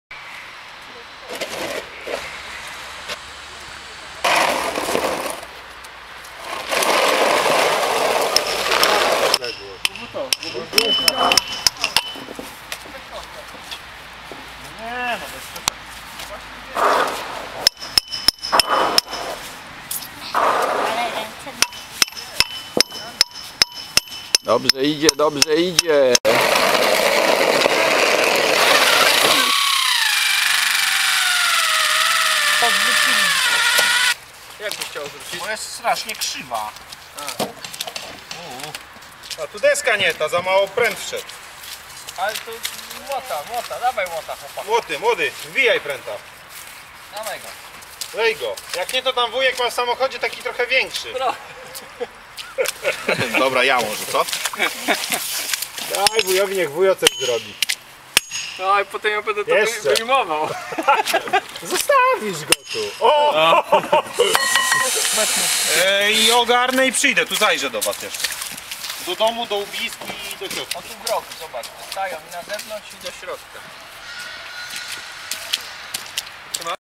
Lodowisko 2017 r Zalesie Górne
Przygotowanie lodowiska w Zalesiu Górnym do zimowego sezonu.